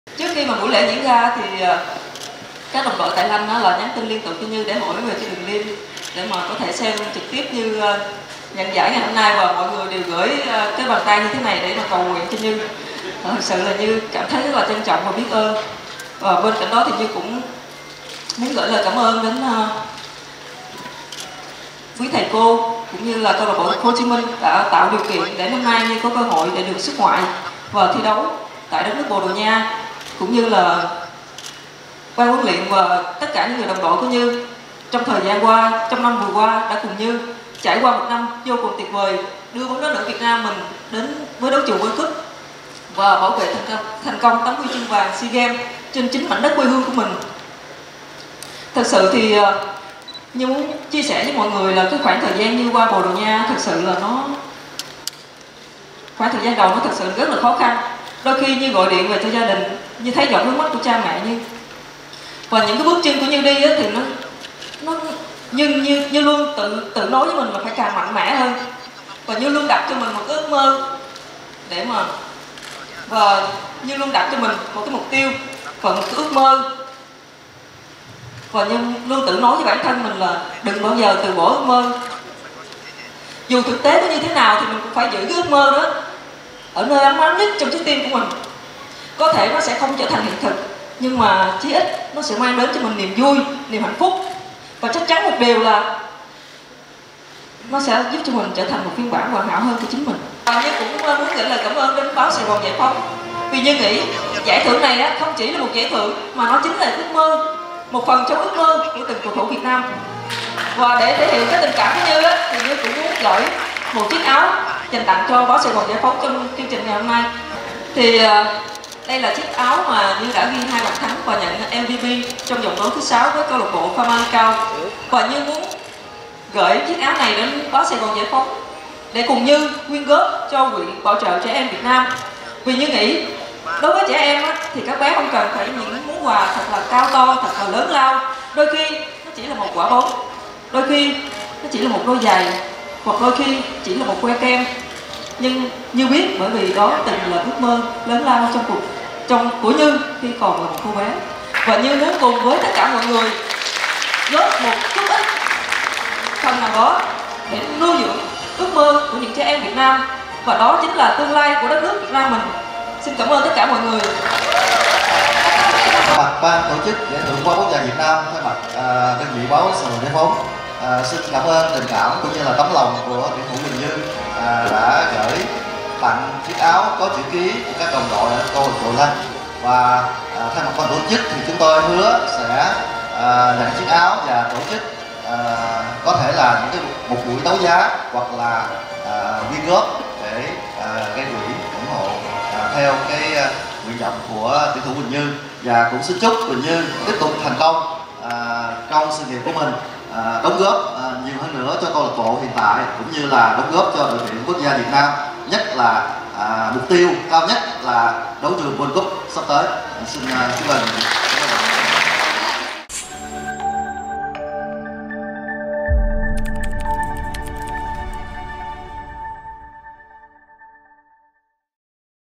Những chia sẻ đầy cảm hứng của Huỳnh Như sau 5 lần đoạt danh hiệu Quả bóng vàng Việt Nam
“Như luôn đặt cho mình một ước mơ, mục tiêu và luôn tự nhủ với bản thân là đừng bao giờ từ bỏ ước mơ dù thực tế có như thế nào mình cũng phải giữ ước mơ đó ở nơi ấm áp nhất trong trái tim của mình. Như muốn gửi lời cảm ơn đến Báo SGGP, vì Như nghĩ giải thưởng này không chỉ là một giải thưởng mà nó chính là một phần trong ước mơ của từng cầu thủ Việt Nam..."